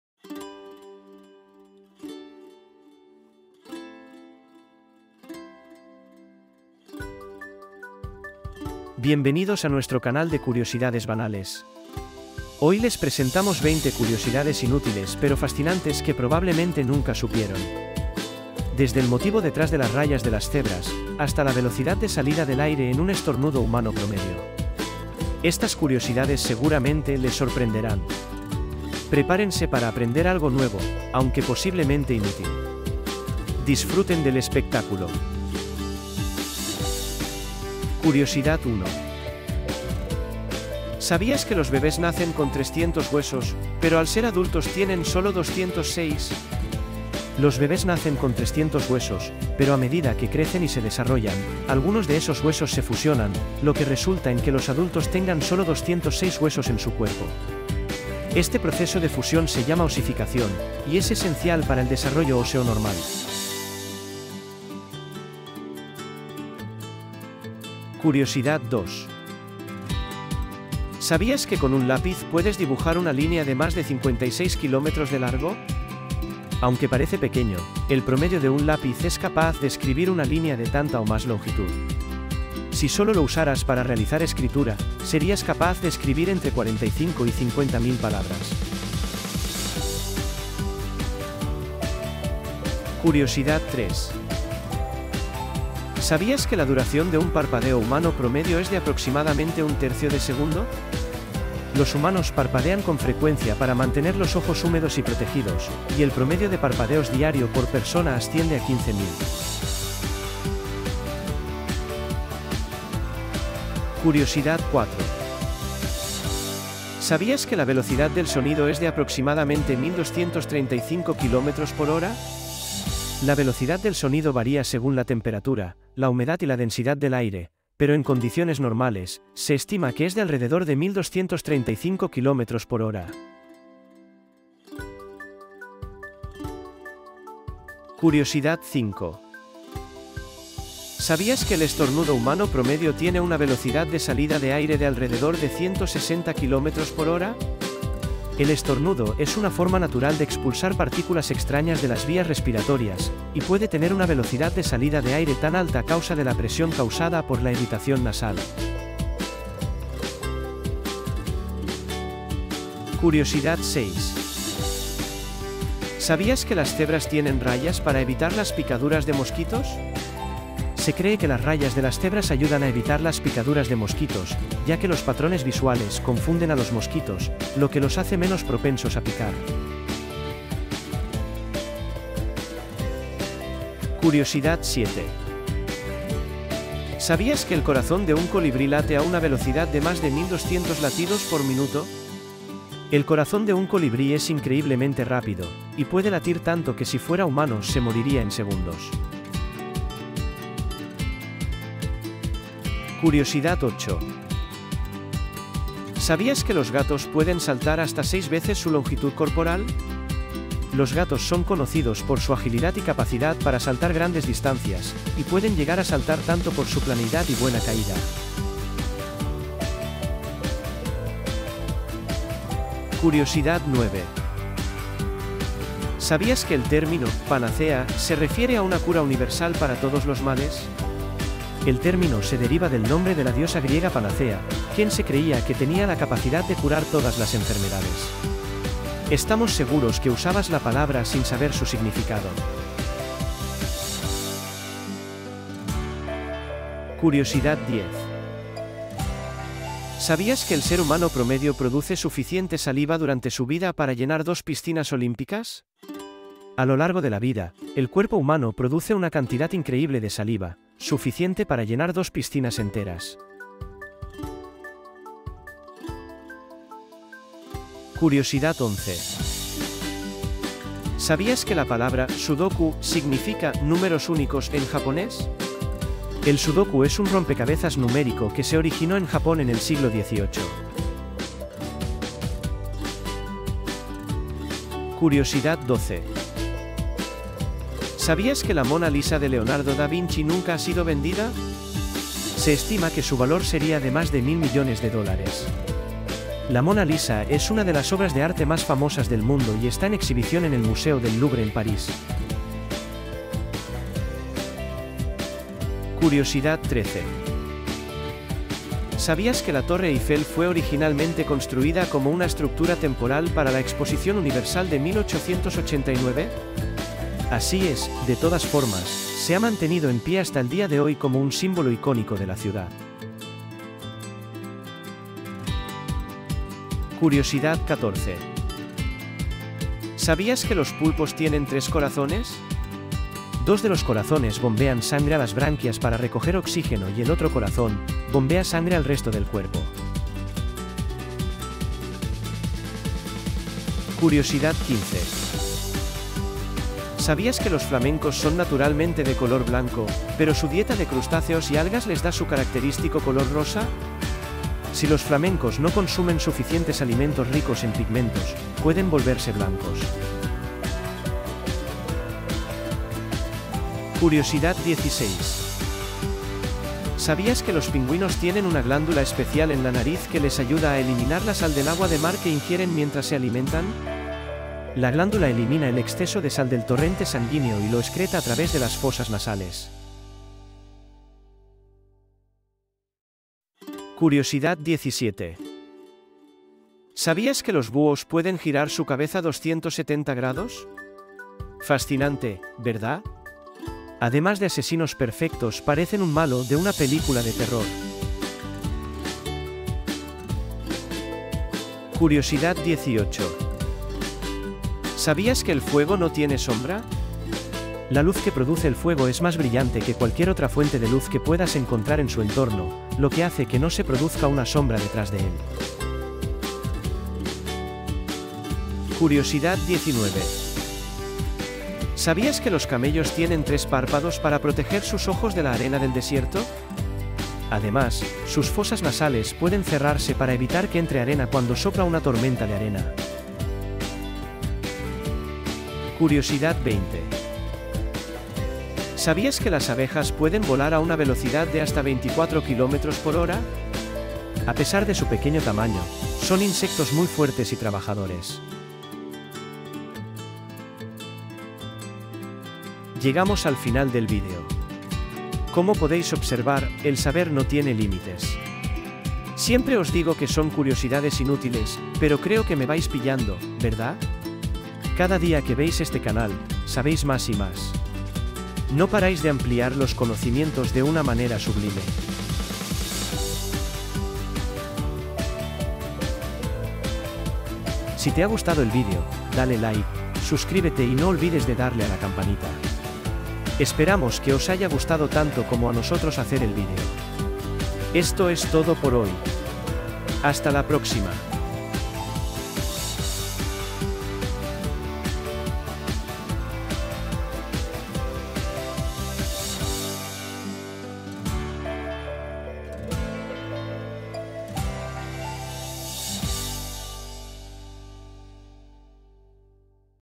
20 curiosidades sobre el mundo que cambiarán tu forma de ver las cosas - Episodio XII
En este vídeo te traemos 20 curiosidades fascinantes sobre animales, naturaleza y seres humanos que probablemente no conocías. Acompáñanos en un viaje por el mundo y descubre datos curiosos y sorprendentes sobre nuestro planeta y sus habitantes.

Desde la habilidad de algunos animales y sus formas asombrosas, hasta los misteriosos comportamientos de la naturaleza y las fascinantes capacidades del cuerpo humano, este vídeo te mantendrá pegado a la pantalla.

Aprende cómo algunas criaturas viven en diferentes condiciones, cómo se adaptan al medio ambiente, y descubre cómo el cuerpo humano puede hacer cosas increíbles.

Si eres amante de la naturaleza y te gusta descubrir cosas nuevas, ¡este vídeo es para ti! Prepárate para aprender y sorprenderte con estas 20 curiosidades sobre animales, naturaleza y seres humanos.

Te pica la curiosidad?